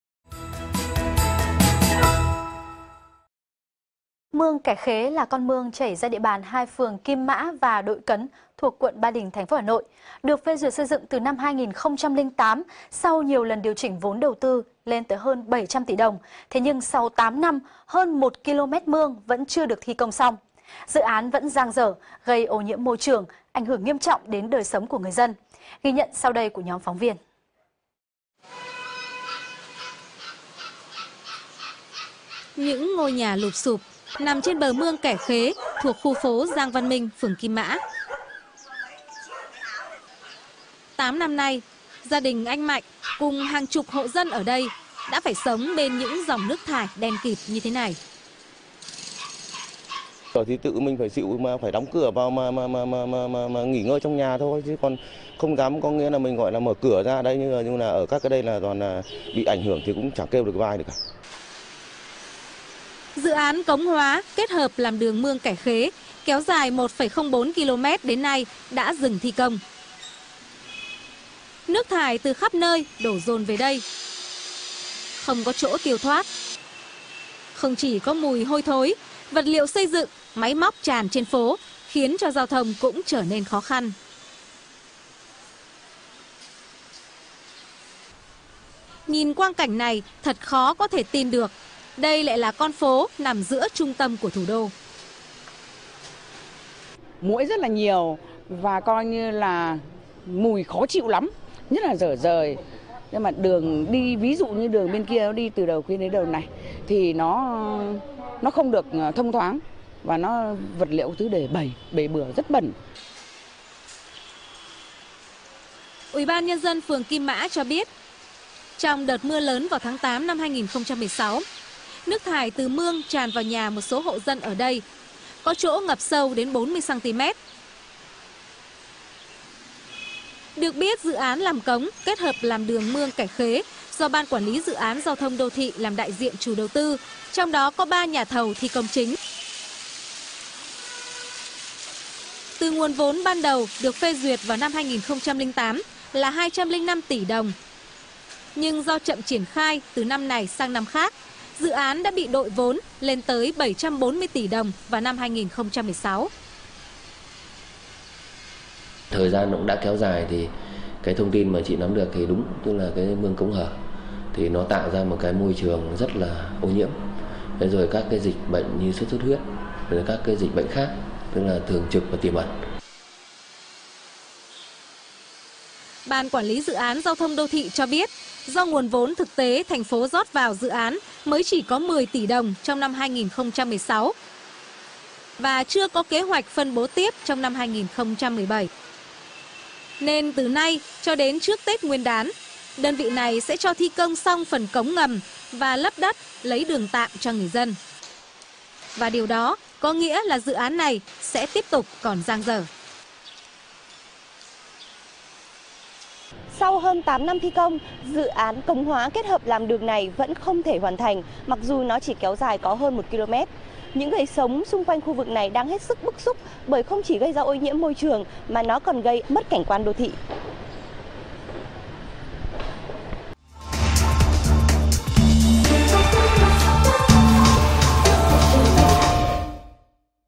VTC Tin Mới
VTC | Mương Kẻ Khế kéo dài 1,04km qua địa bàn 2 phường Kim Mã và Đội Cấn (quận Ba Đình), dự án công hóa dang dở 8 năm khiến người dân bức xúc vì ô nhiễm môi trường và mất cảnh quan đô thị.